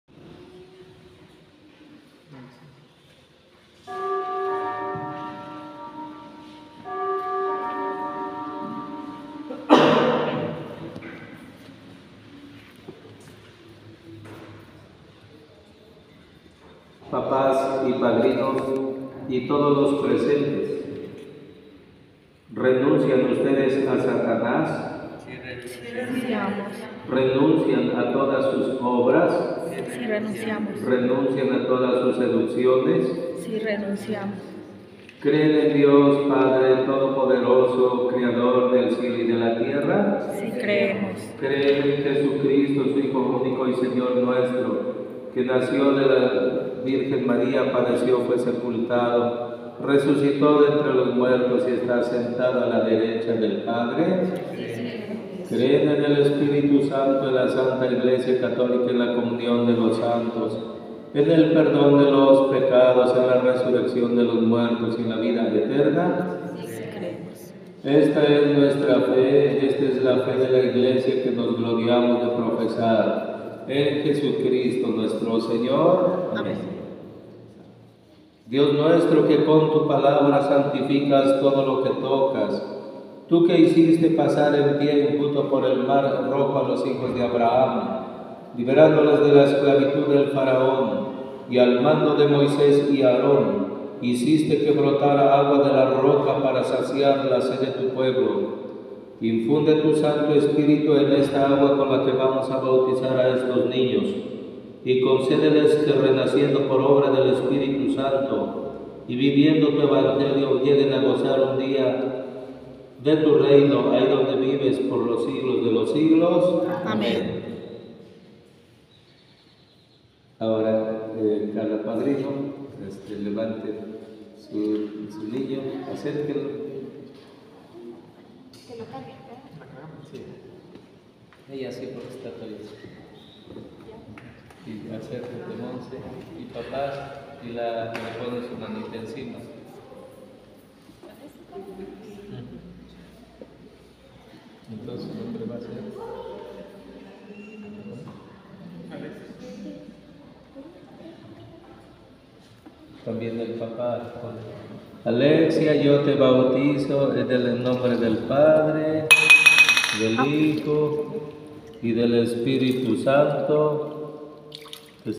la nenavlogs sigue la celebración del bautizo de Luis y Alexia
así están muy felices contentos